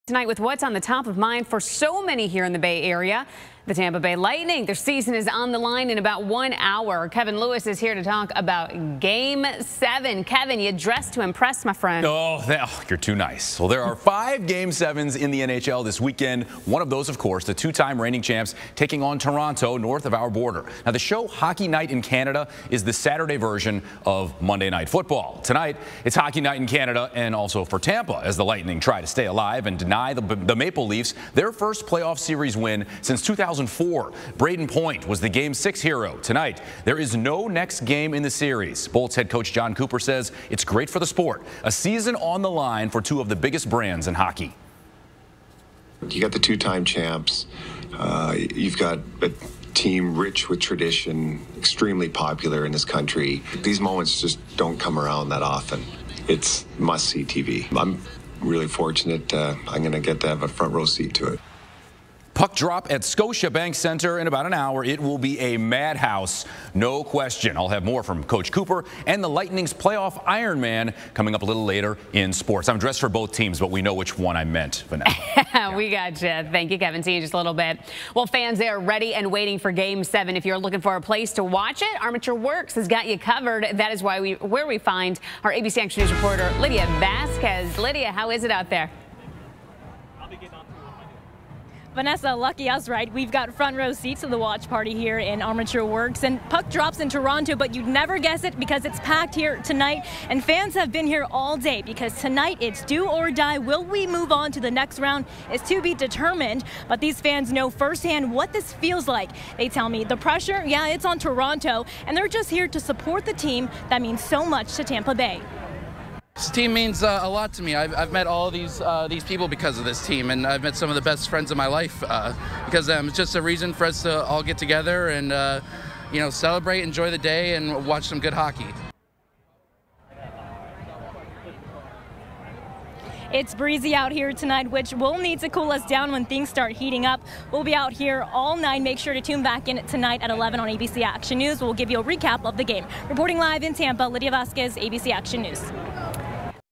Lightning game 7
Bolts ready for game 7 of Stanley Cup Playoffs